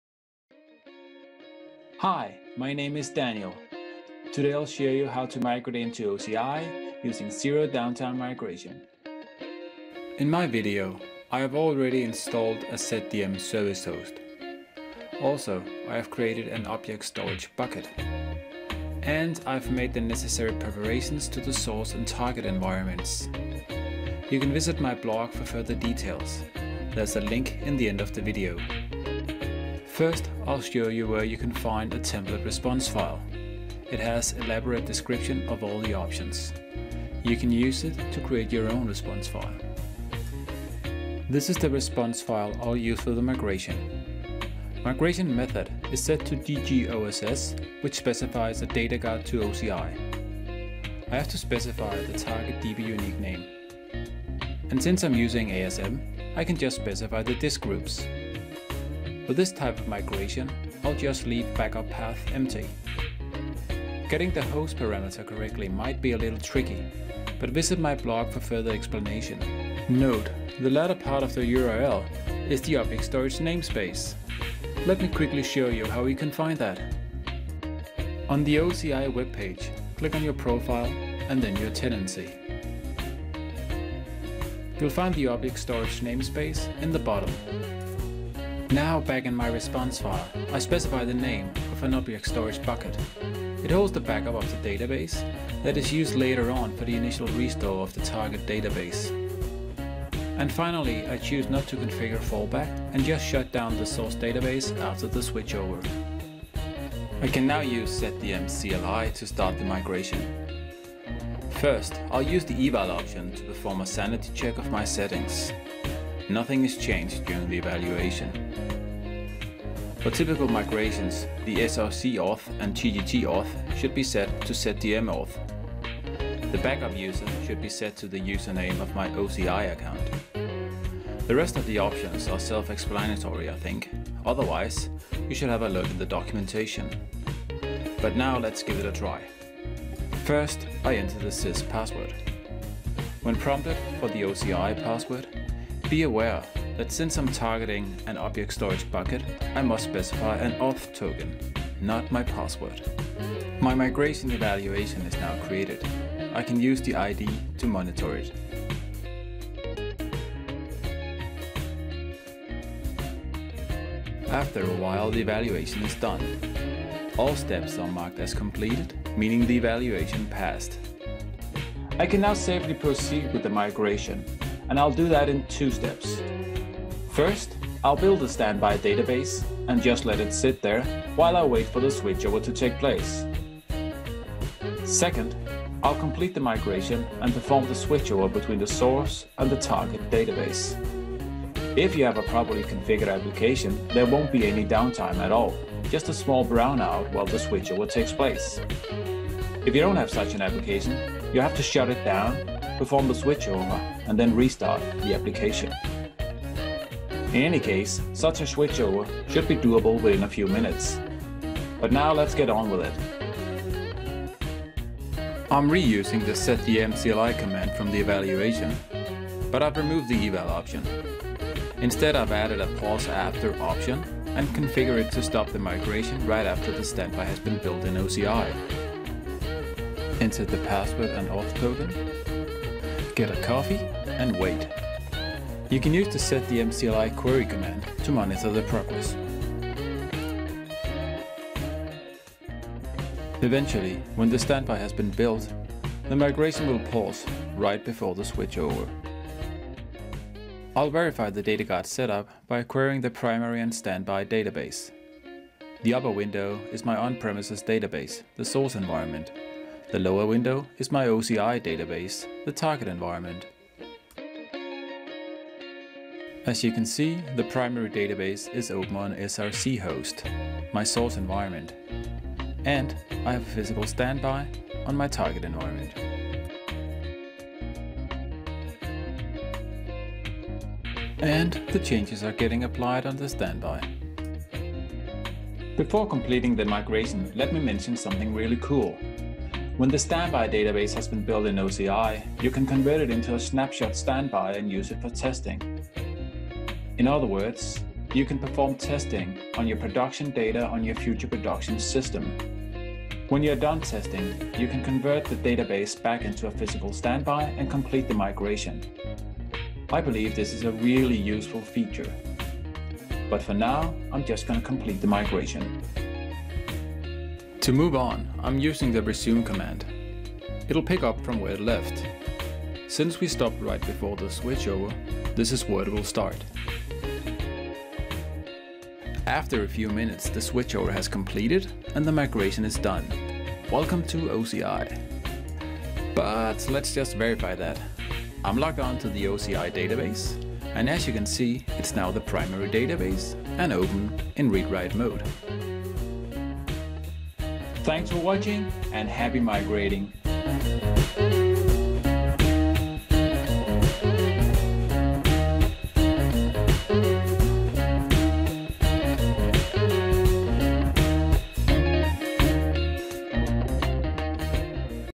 Zero Downtime Migration (ZDM) - Migrate to OCI
In this video, I will show you how to migrate a database into Oracle Cloud Infrastructure (OCI) using Zero Downtime Migration (ZDM).